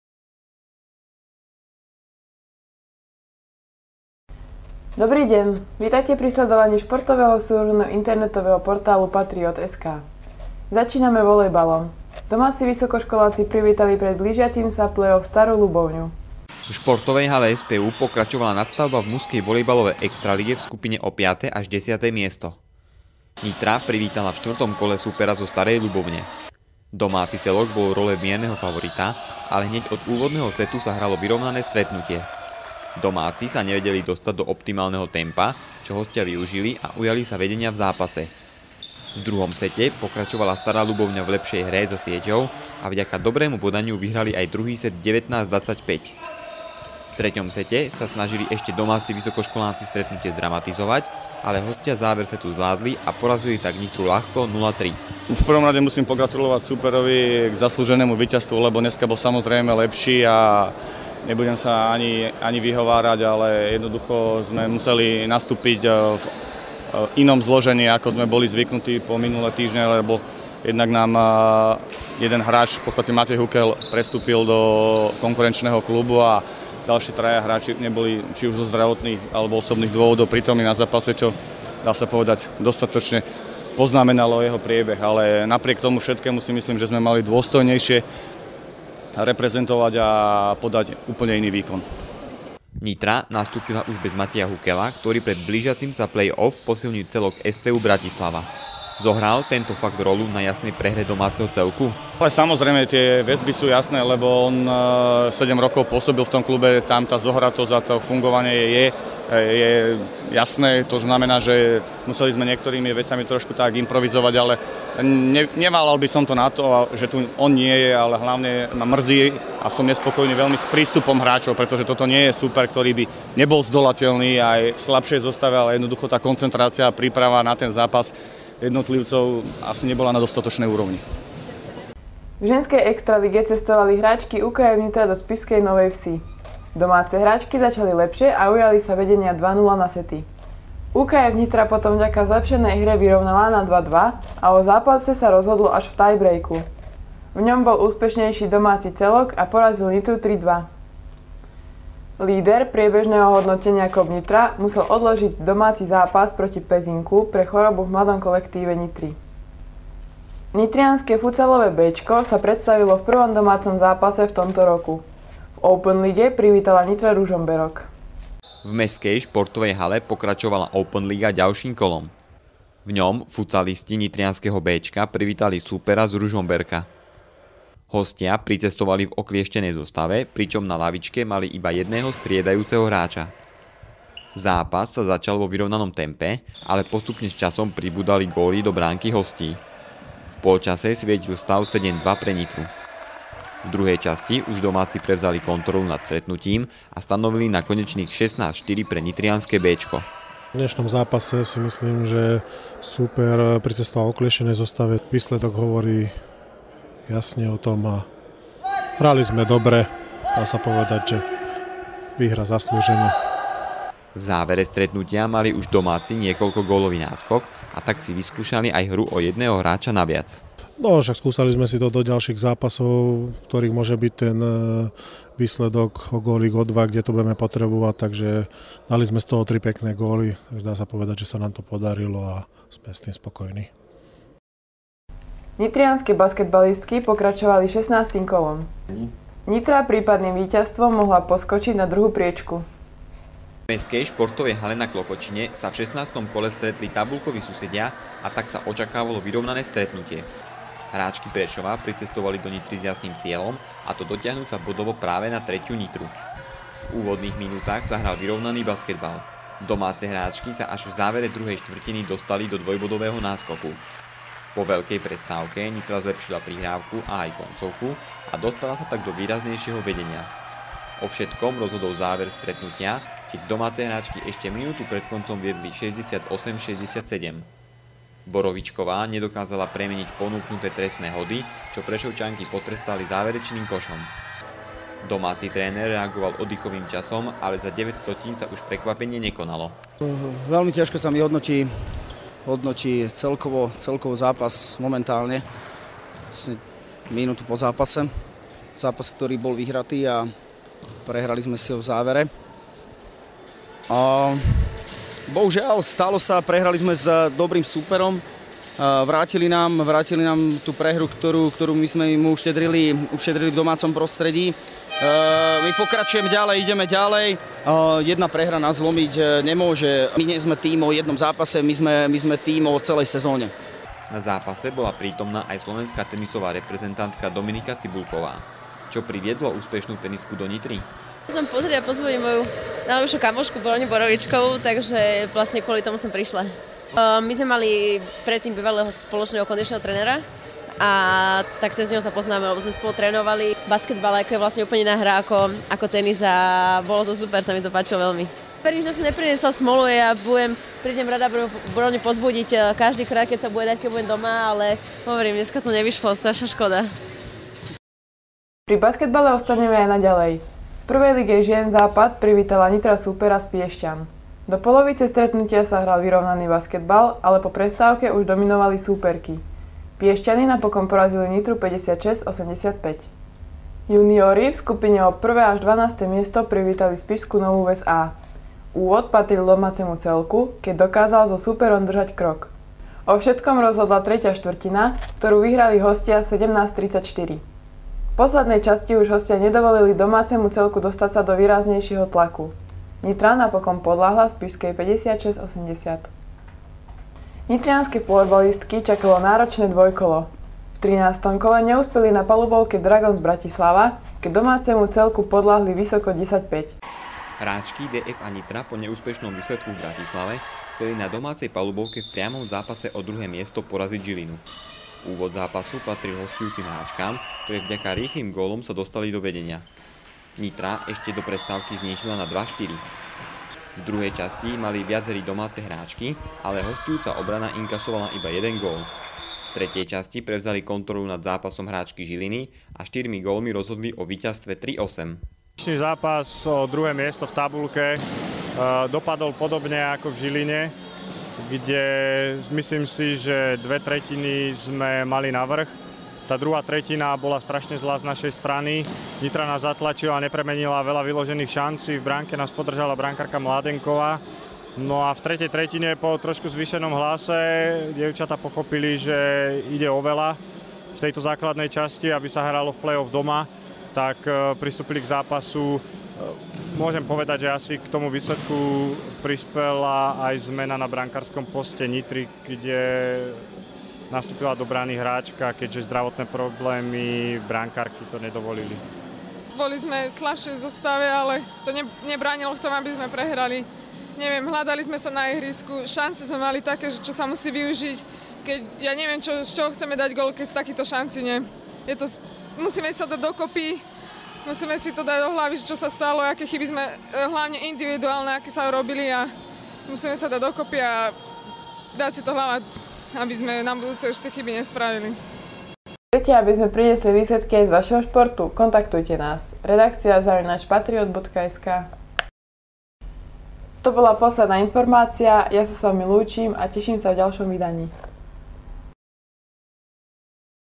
Patriot: Súhrn najdôležitejších športových udalostí v Nitre - týždeň 03/2011
Prinášame Vám súhrn najdôležitejších športových udalostí v Nitre za
3. týždeň roku 2011. Každý deň sa dočítate aktuálne informácie a výsledky zo
športových stretnutí.Teraz máte možnosť pozrieť si sumár toho
najdôležitejšieho z nitrianskeho športu. Ak chcete aby sme prinášali